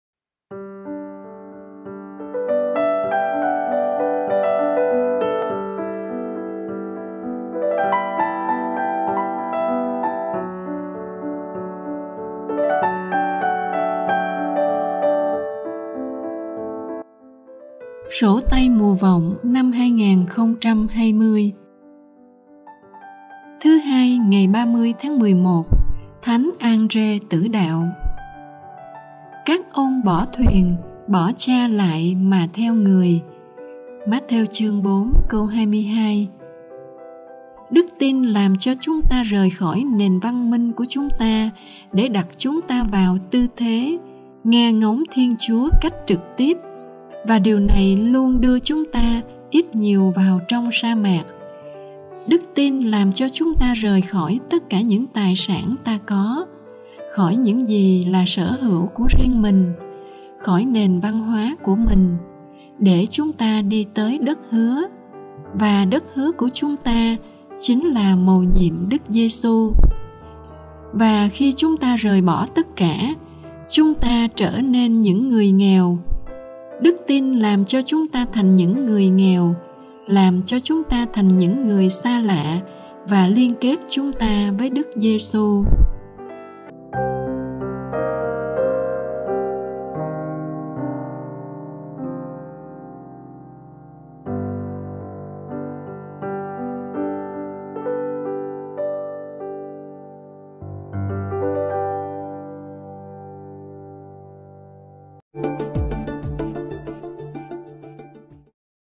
SỔ TAY MÙA VỌNG 2020 (Số 2)
Thứ Hai, ngày 30 tháng 11 - Thánh Anrê, tử đạo
"Các ông bỏ thuyền, bỏ cha lại mà theo Người" (Mt 4, 22)
Nguồn: Đan Viện Dòng Đức Mẹ Sao Mai